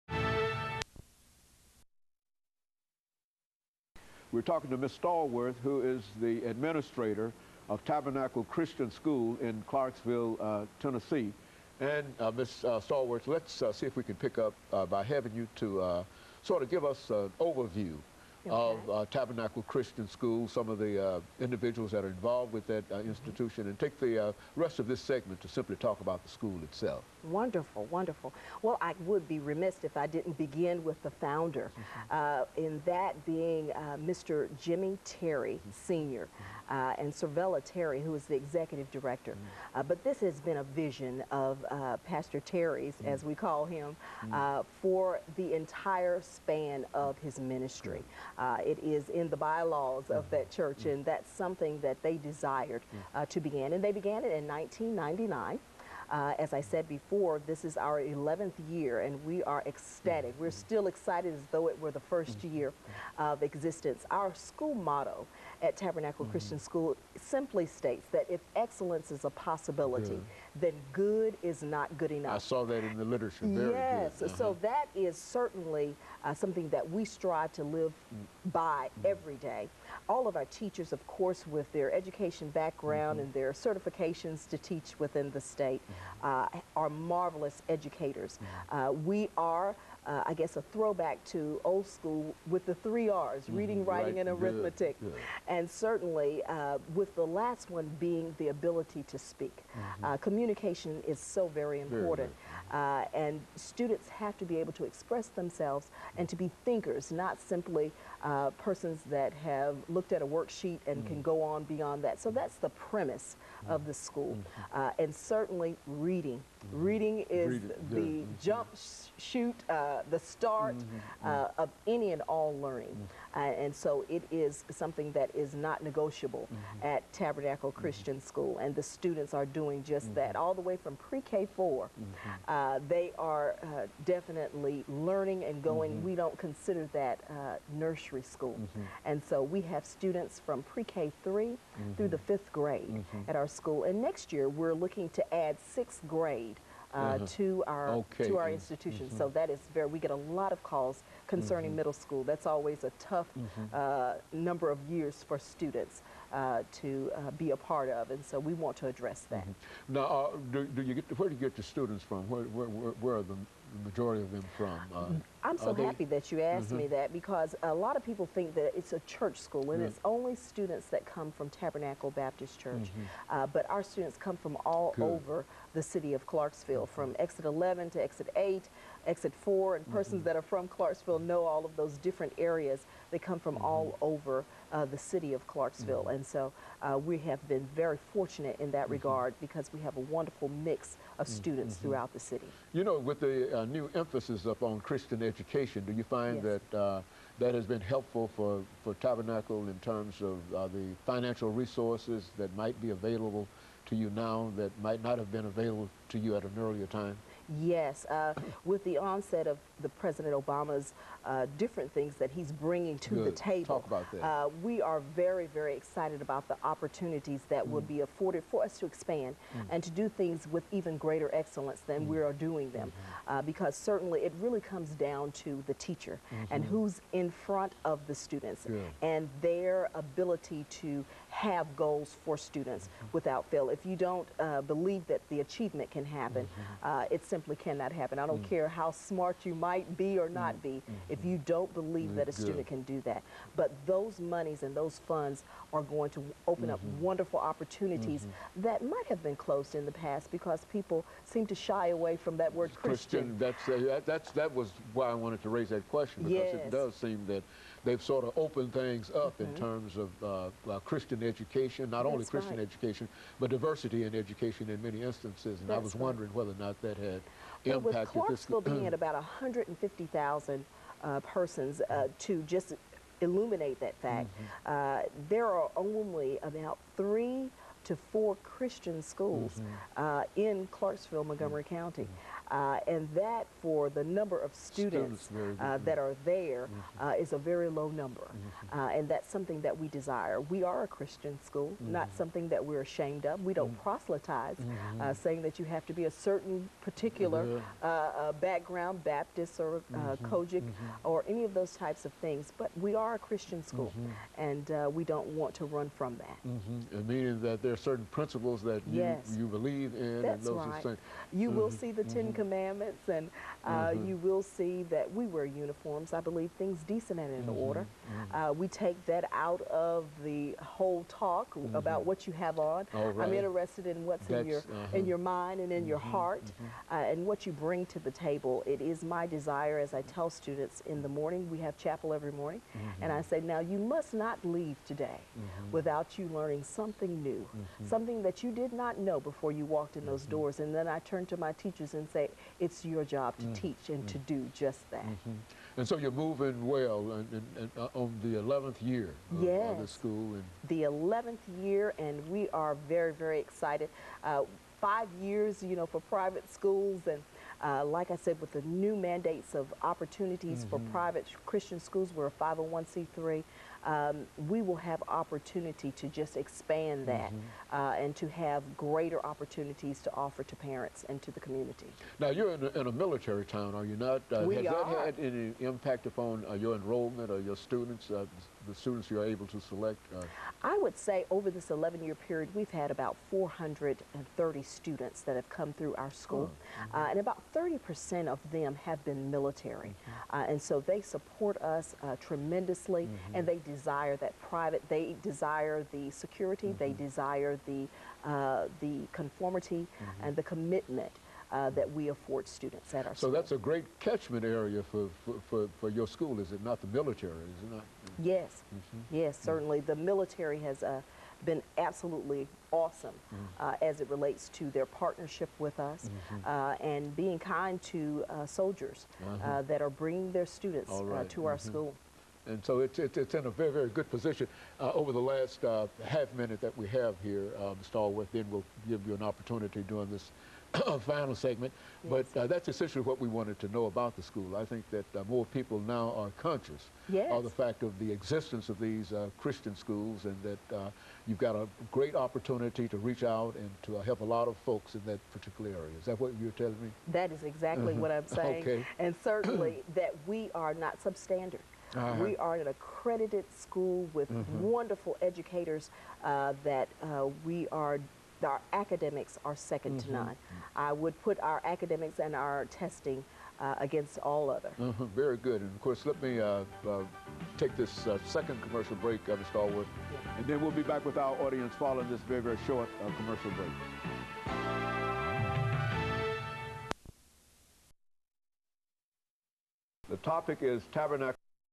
Christian Schools, KStallworth2
Comments With Dr. James Haney Presents*Christian Schools, with KStallworth, an administrator at a Christian School, who talks about some of the positive changes that have occured toward Christian schools lately, and how these schools provide an excellent option to public schools, Part 2. Juliet, Tn, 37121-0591. I appreciate any help you can give, drjhaney

(more)